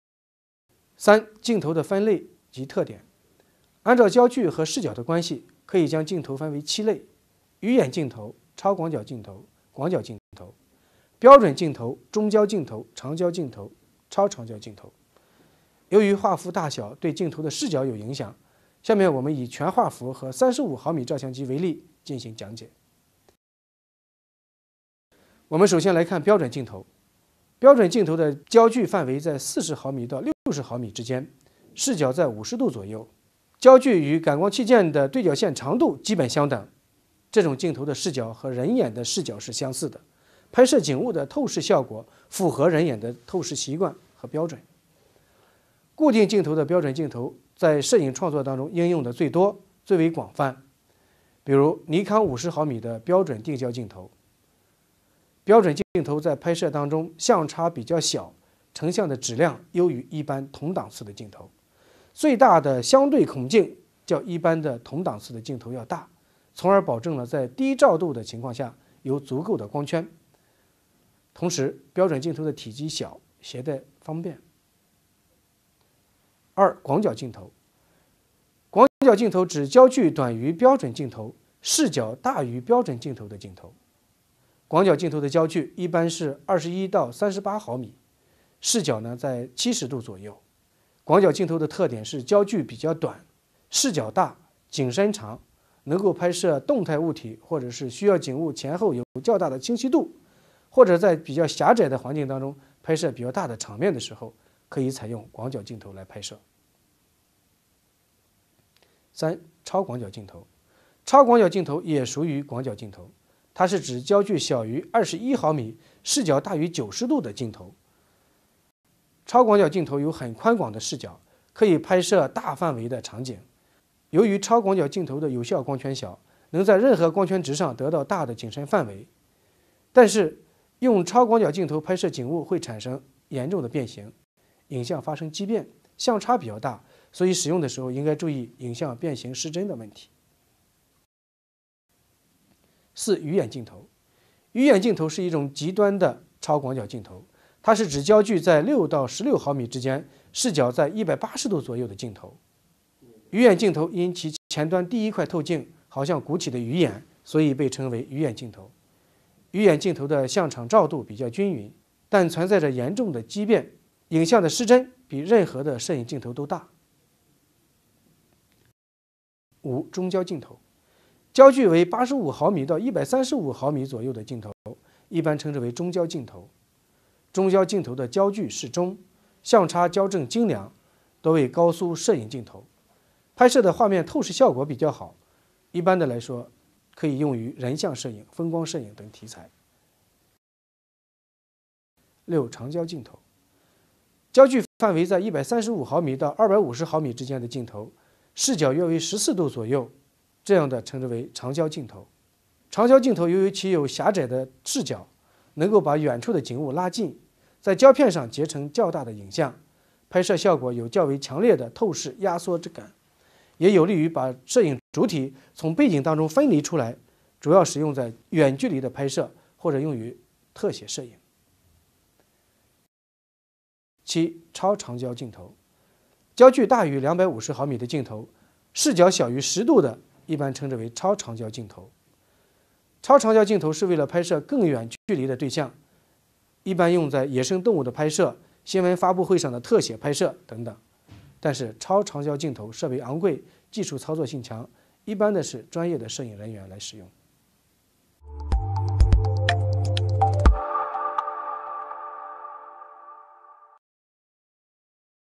数码摄影艺术与技法 | 3.3 镜头的分类及特点
本课程从摄影和摄像美学原理出发，分别讲解数码单反照相机的性能结构、镜头造型、曝光原理、光线知识、色彩原理、景深原理、构图知识以及摄影历史、本质和社会功能等，课程内容还包括摄影技能（如：对焦、用光、运动拍摄、景深控制、构图技巧等）和主题摄影（如静物、风光、人像、广告、新闻、体育、纪实等题材拍摄）